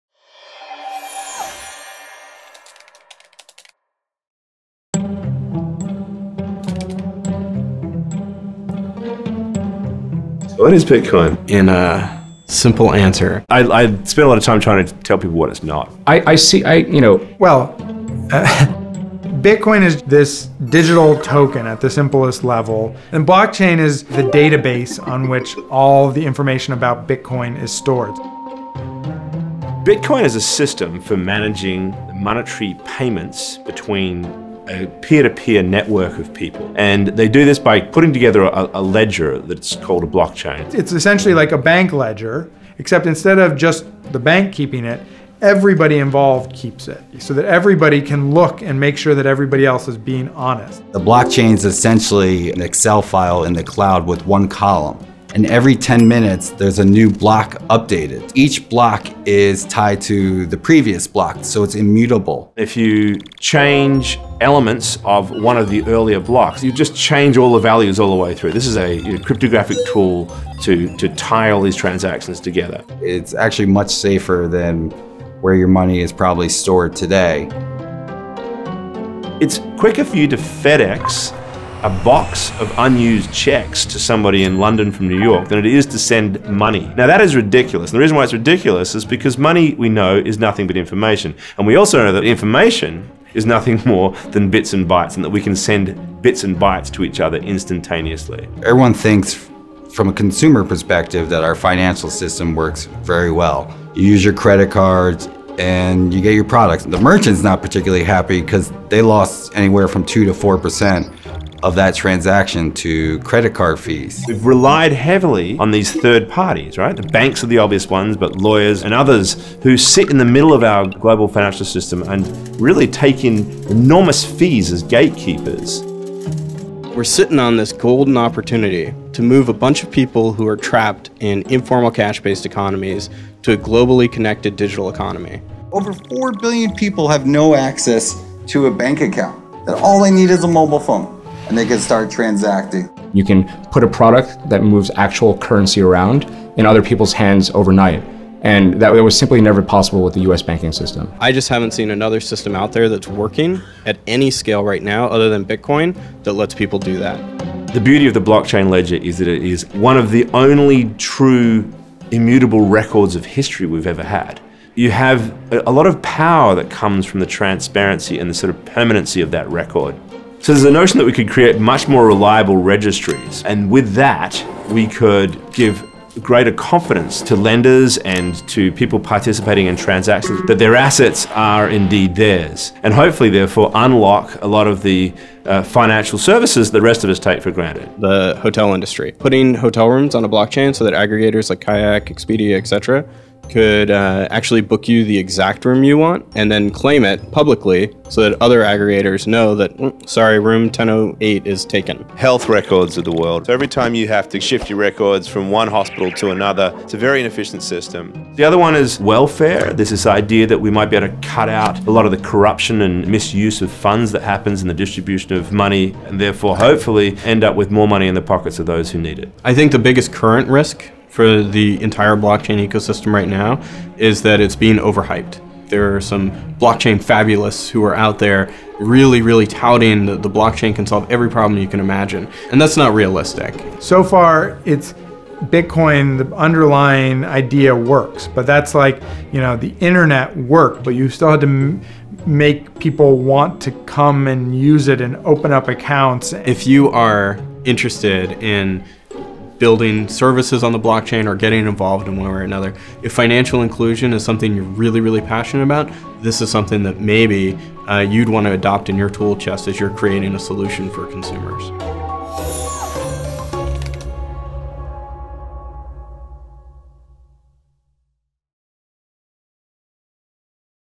Bitcoin & the Blockchain Primer
Recently, the organization Harvard in Tech, the official Harvard alumni group for technology, hosted a panel of experts on bitcoin and the blockchain. We were thrilled to attend the event, where we learned some of the basics of this cryptocurrency, and its potential impact on the global financial system. While there is a great deal more to bitcoin and the blockchain than we were able to explore in this piece, we hope it serves as a useful primer on some key bitcoin concepts, and makes the case for why we should be paying attention to developments in the field.

We want to thank Harvard in Tech, and the following panelists:

Nathanial Popper: Business Reporter at The New York Times
Michael Casey: Senior Advisor on Blockchain Opportunities, MIT Media Lab’s Digital Currency Initiative
Chris Lavery: Vice President of Operations & Finance at Blockchain
Patrick Murck: Lawyer & Fellow at the Berman Center for Internet & Security at Harvard
Paul Chou: CEO & Co-Founder at LedgerX